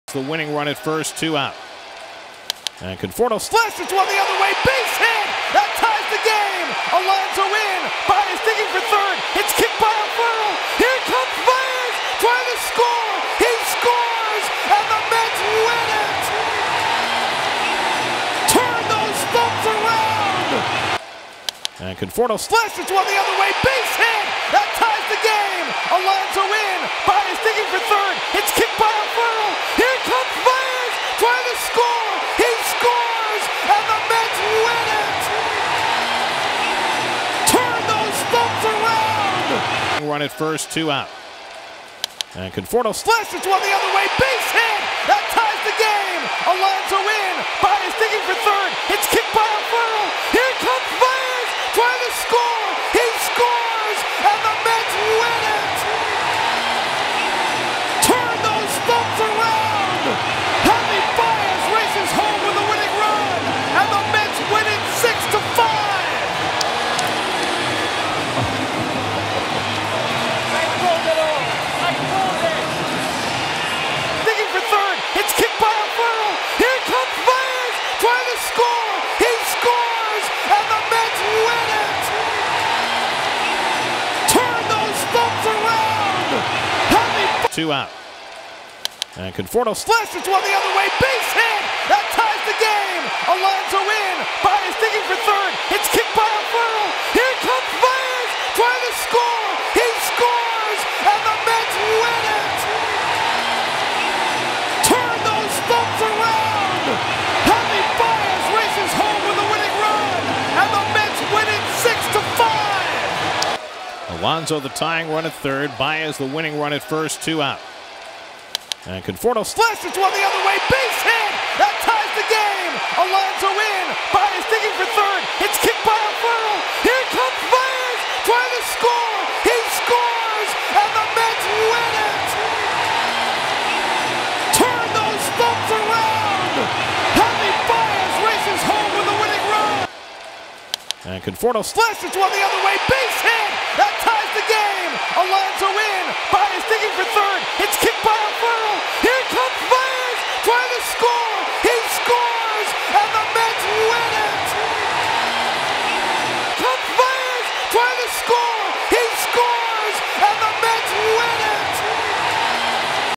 Michael Conforto's walk-off hit, Javier Baez's slide from every angle | Mets vs Marlins | SNY
Check out every angle of Michael Conforto's bottom of the 9th 2-RBI walk-off and see Javy Baez go first to home to lead the New York Mets to a 6-5 win over the Miami Marlins.